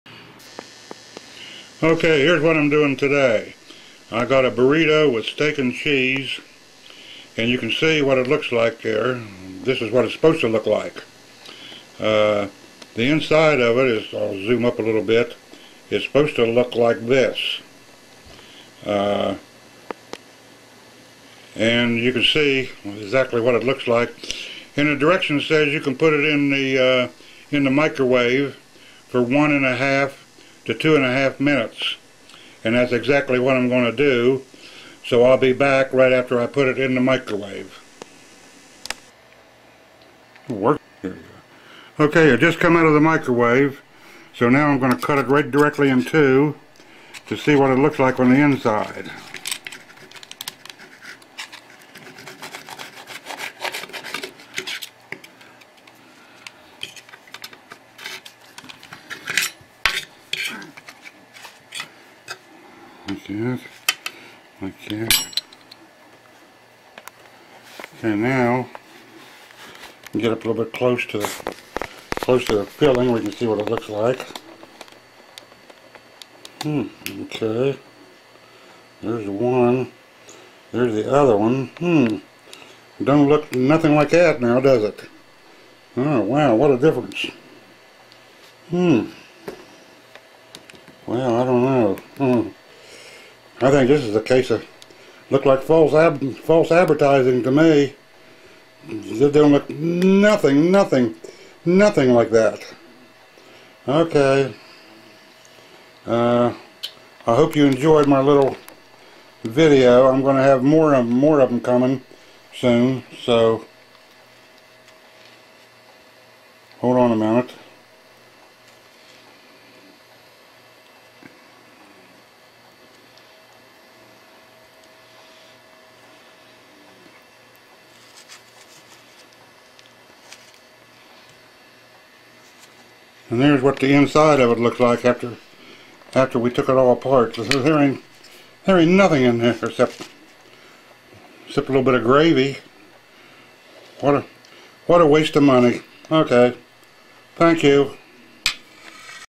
Burrito's---Lets look inside the wrapper
Jose Ole---Burrito, with Steak & Cheeze
Here's what they really look like in side the wrapper.